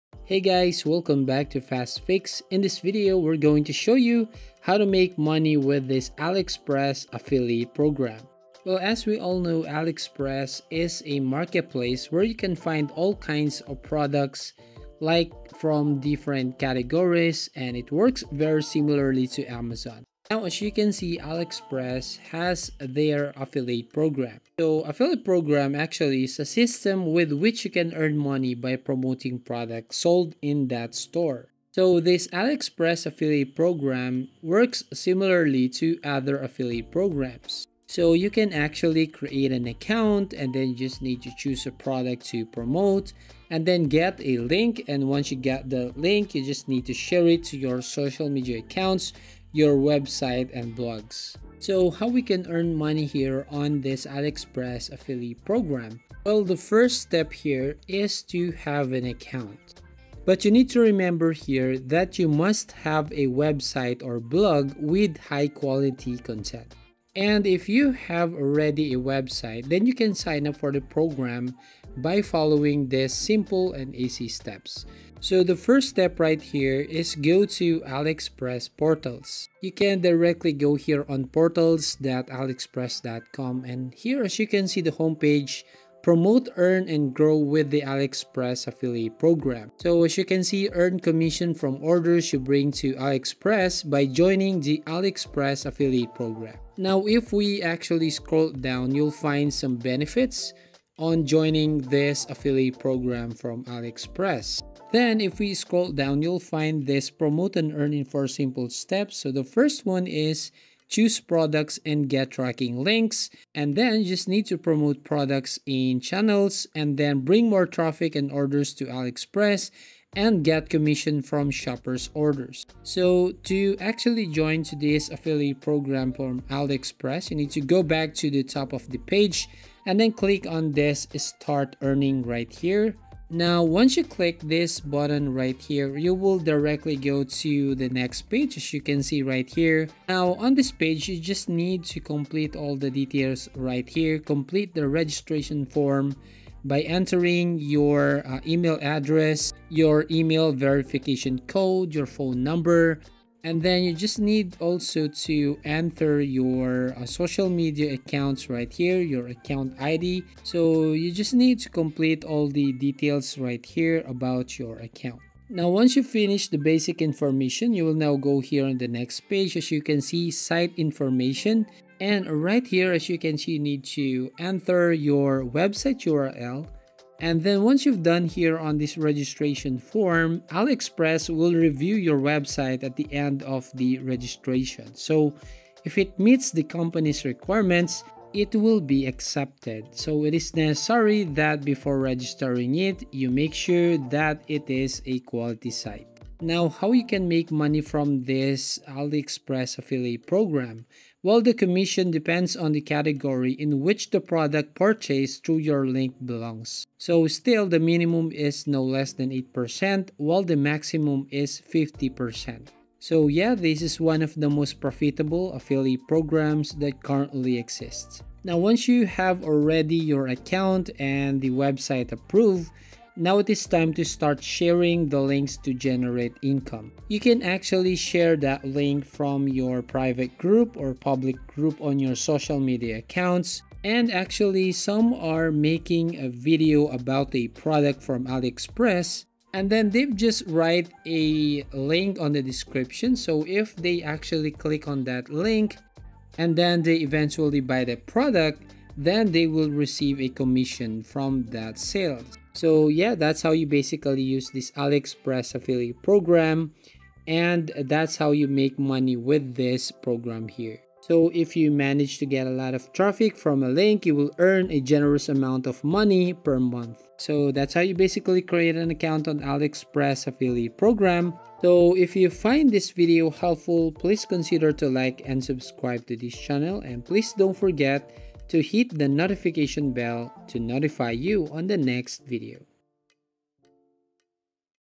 🔥 ALIEXPRESS Affiliate Program Tutorial | How To Make Money From AliExpress 2023
In this video I'll show you the AliExpress Affiliate Program Tutorial | How To Make Money From AliExpress 2023. The method is very simple and clearly described in the video. Follow all of the steps & you will know how to make money with Aliexpress affiliate program!